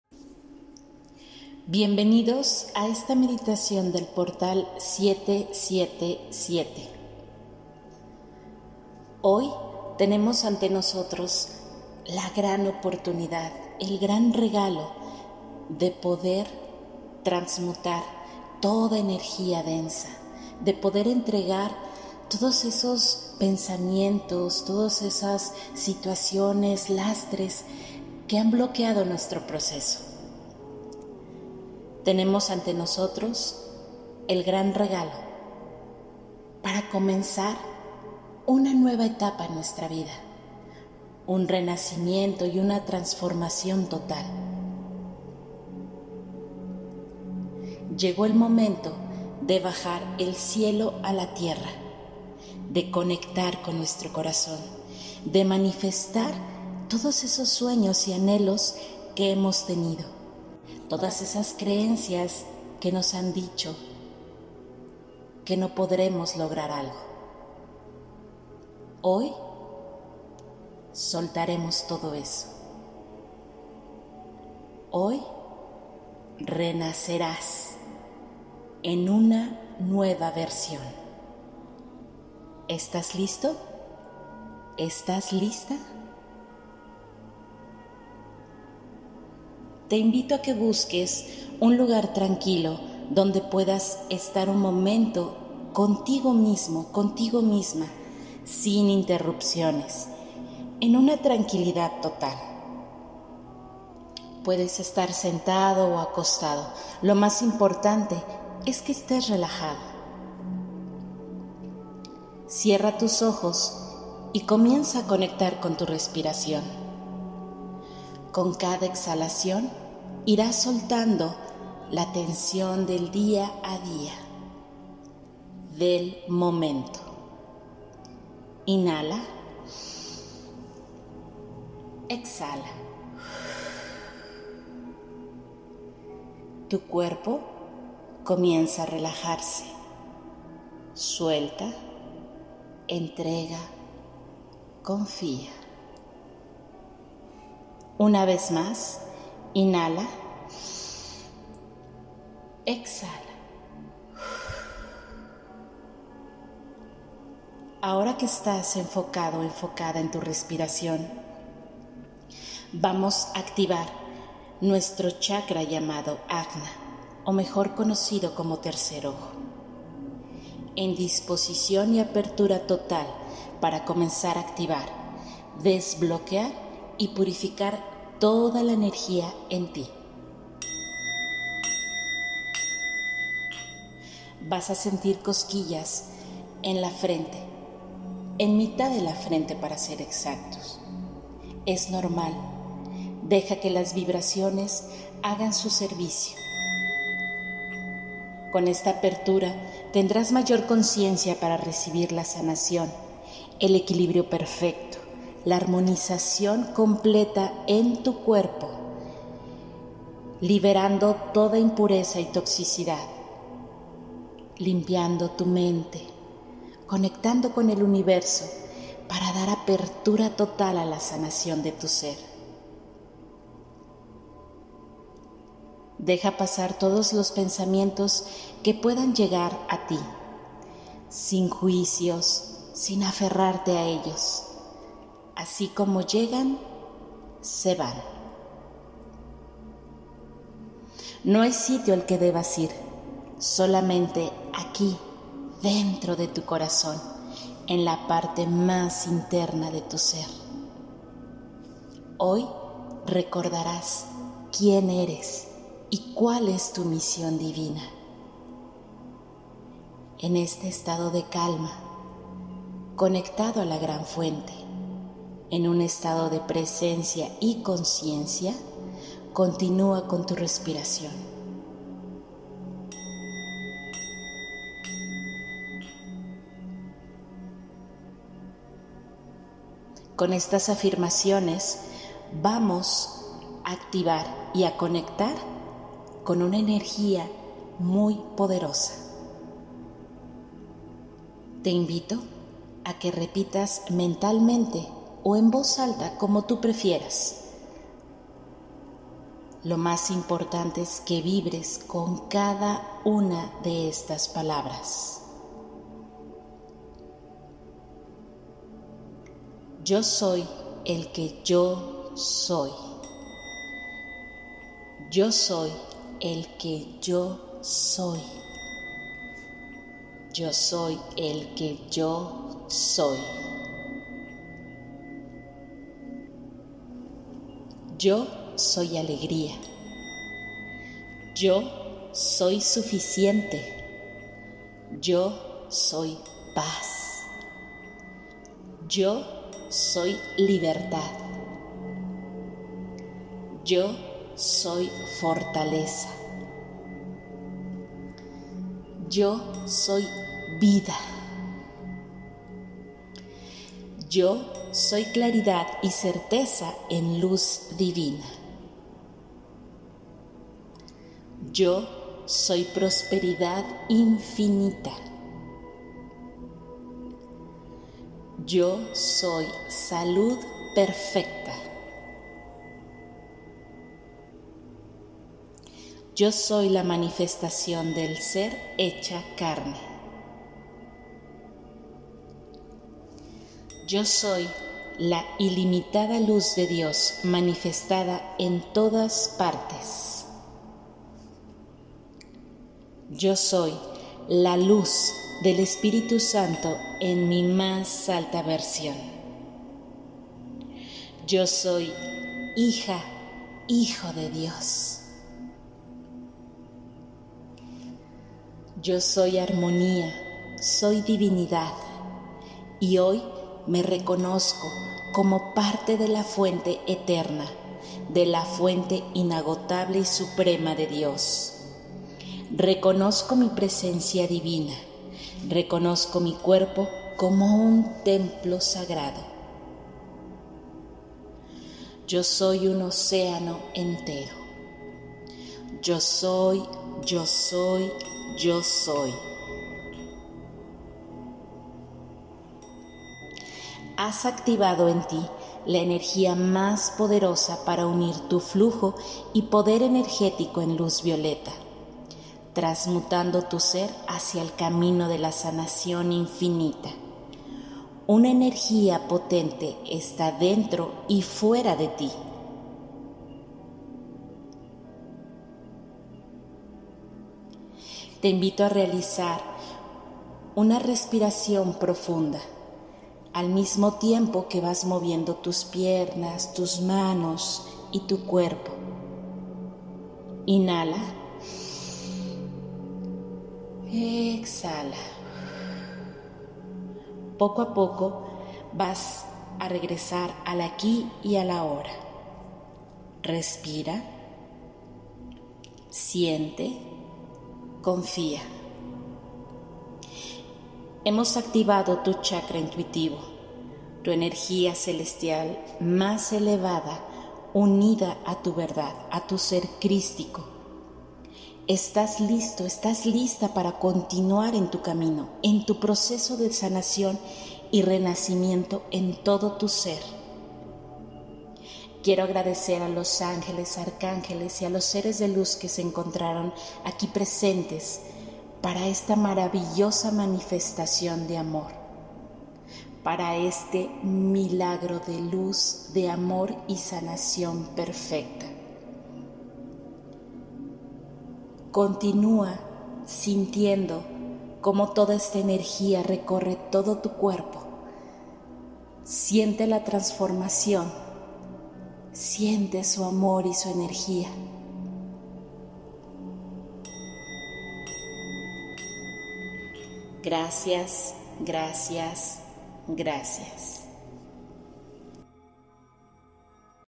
🕉️ MEDITACIÓN PORTAL 777 | SALTO CUÁNTICO✨ PROSPERIDAD INFINITA 💎
Te invitamos a escuchar esta poderosa meditación para activar tu tercer ojo, para alinear tus Campos energéticos y recibir la sanación total. El despertar de conciencia, se une a la energía más elevada del" YO SOY", realiza esta meditación por 21 días para que sus efectos en ti, te generen mayor conexión y apertura.
Que el poder y la permanencia en tu espíritu sea infinito.

Namaste
Gracias por tu tiempo y atención espero que todos los mensajes sean alimento para tu alma.

Para sanar hay que sentir y cuándo sientes,la enfermedad desaparece porque la reconoces y la liberas